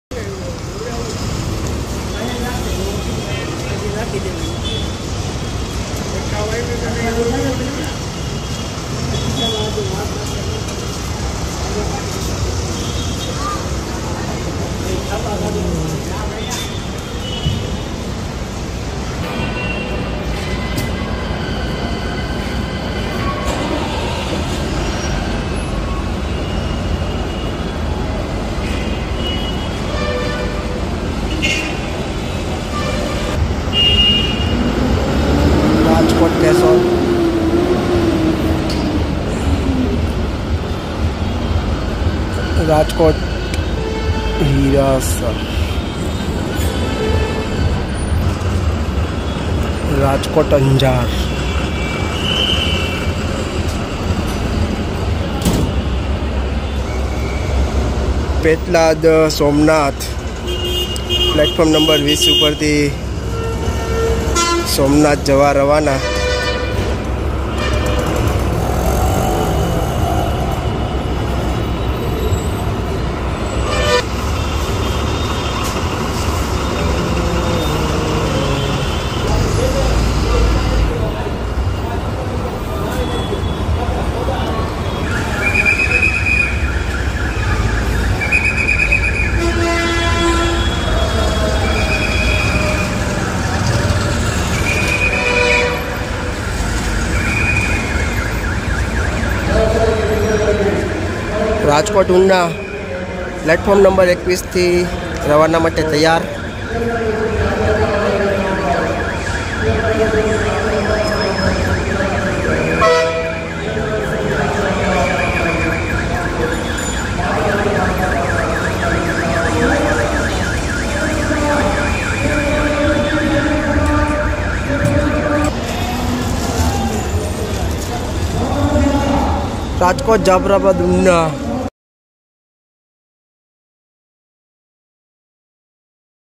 બપોરે 01:04 રાજકોટ બસ સ્ટેન્ડ થી રાજકોટ-કેશોદ | રાજકોટ-ઉના | પેટલાદ-સોમનાથ | રાજકોટ-અંજાર જતી બસો
1.rajkot to keshod
2.rajkot to anjar
3.rajkot to hirasar
4.rajkot to una
5.rajkot to una via jafrabad